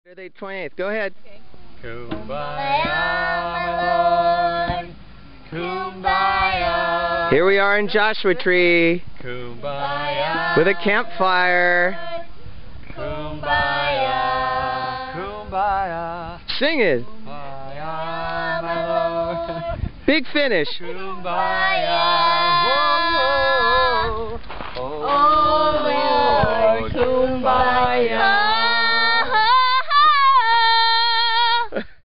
Joshua Tree 2005
We introduced my niece Kira and her dad Kent to Joshua Tree National Park in 2005. Here is a video of the extremely shy Kira experiencing her first campfire in the desert.